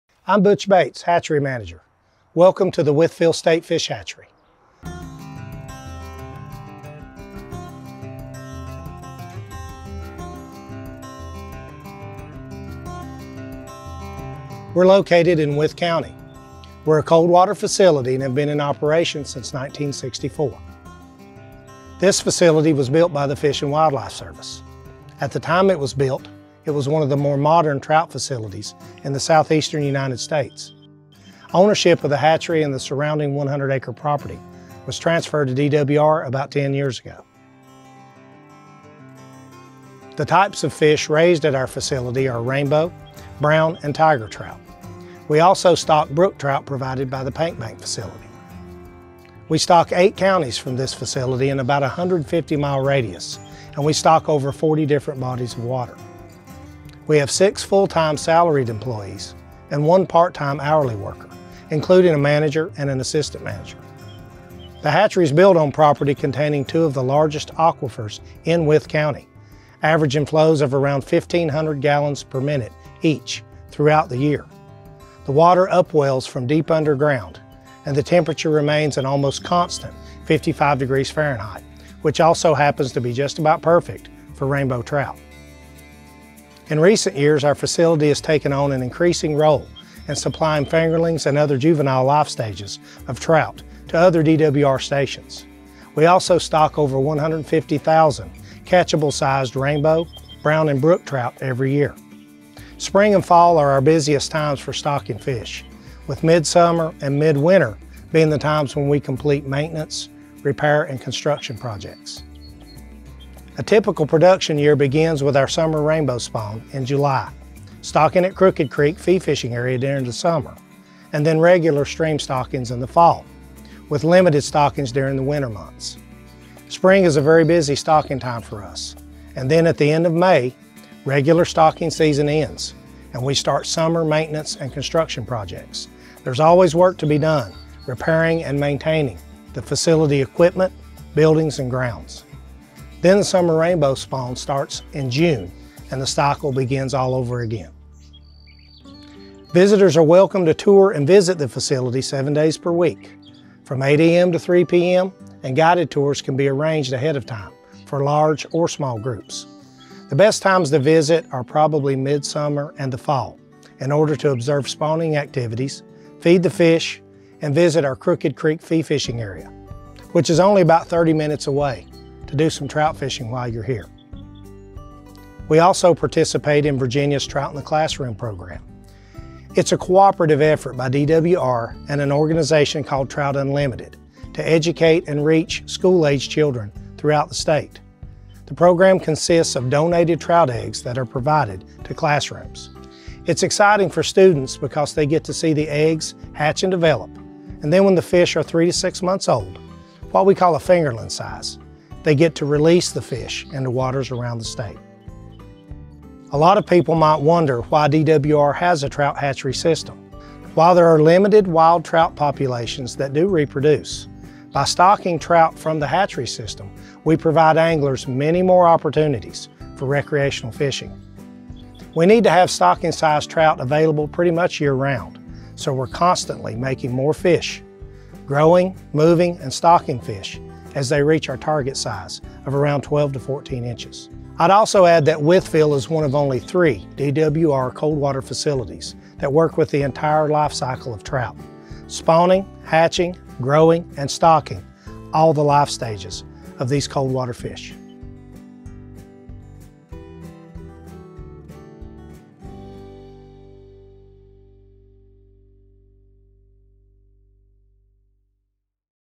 Wytheville Fish Hatchery
Located in Wythe County, Virginia, the Wytheville Fish Hatchery is a premier cold-water facility that has been raising rainbow, brown, and tiger trout since 1964. This hatchery plays a vital role in Virginia's trout stocking program, supporting over 40 bodies of water across eight counties.

Powered by two of the largest aquifers in the region, it maintains an ideal water temperature of 55°F year-round for trout production. The hatchery also contributes to Virginia’s Trout in the Classroom program, educating future generations about fish conservation.

Discover how Wytheville Hatchery manages every stage of the trout life cycle, from spawning to stocking, and learn about the hard work behind supporting recreational fishing in Virginia.